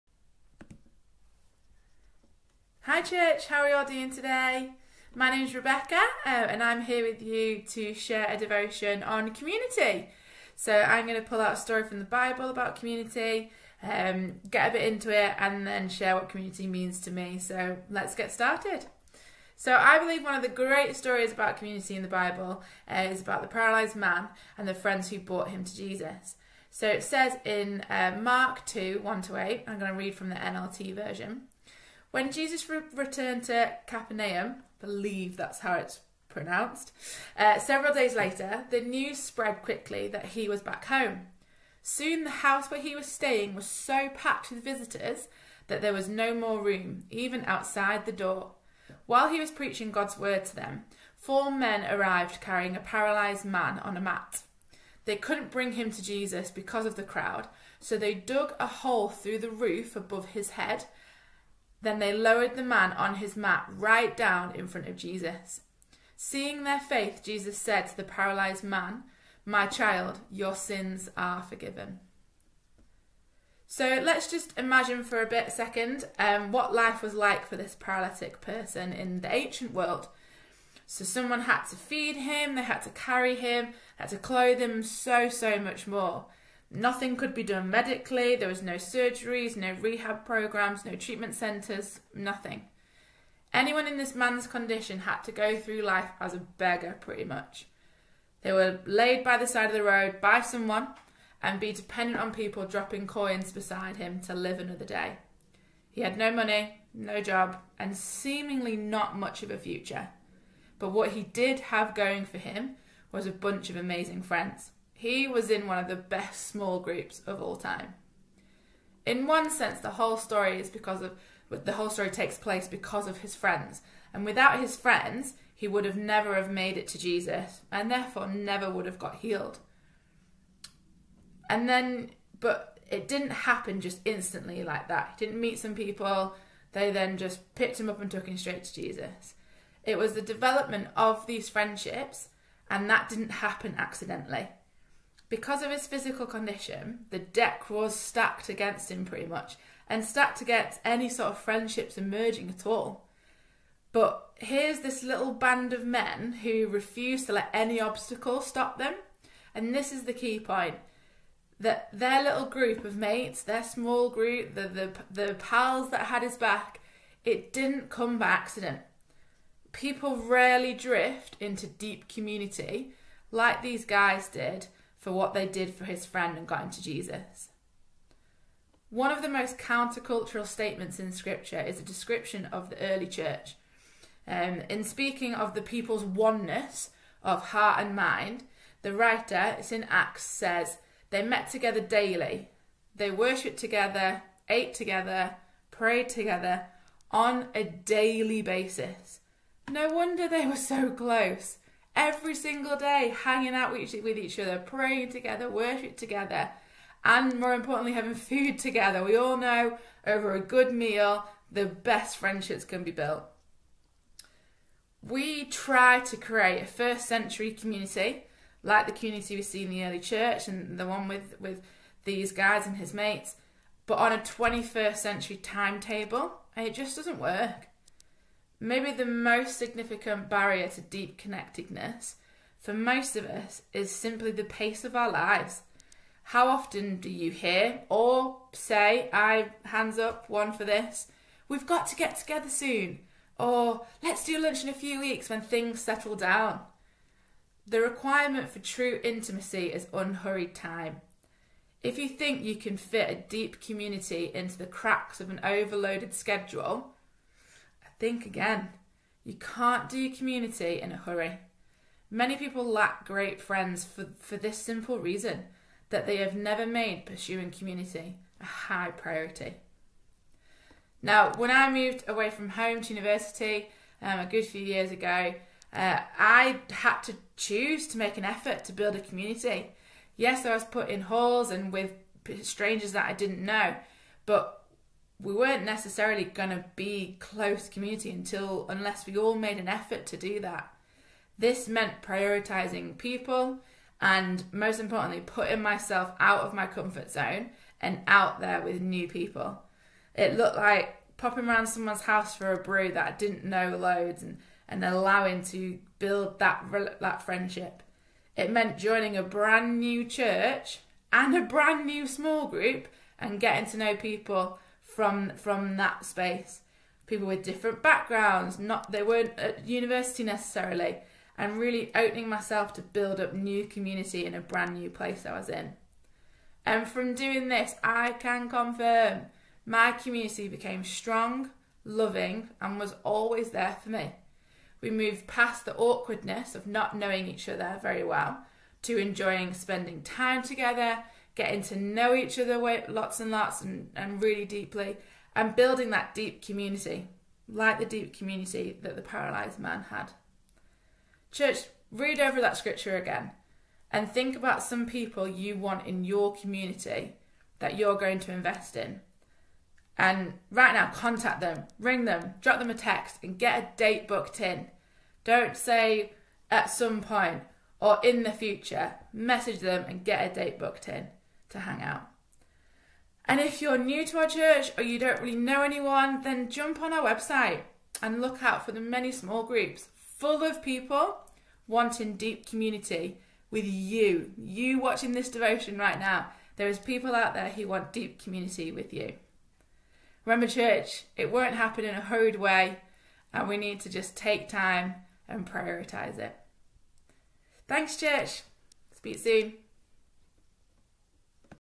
!Audacious Devotionals: Relationships - Thursday 15th June 2023
Today we continue our series of Devotions based on Relationships where a number of people from Audacious Church share stories about their relationships.
Todays devotion is from Rebecca Davison who is part of our Central Manchester Campus.